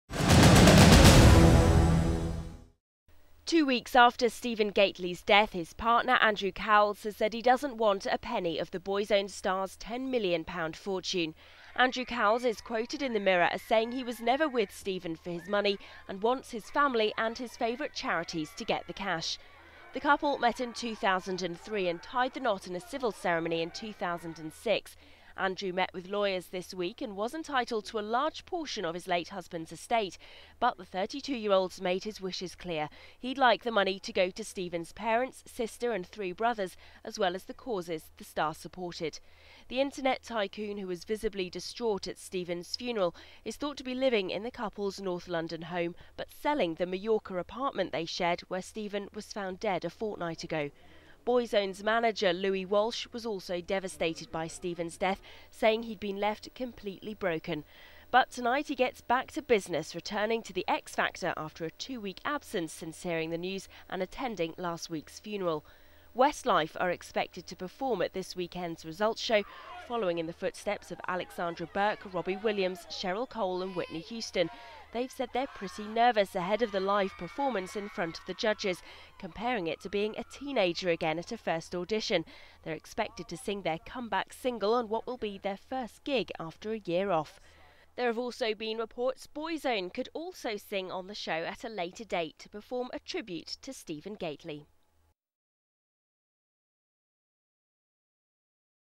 Gately's partner doesn't want 'a penny'
Stephen Gately's partner Andrew Cowles has said he doesn't want any of the star's £10 million fortune as Louis Walsh prepares for X Factor return.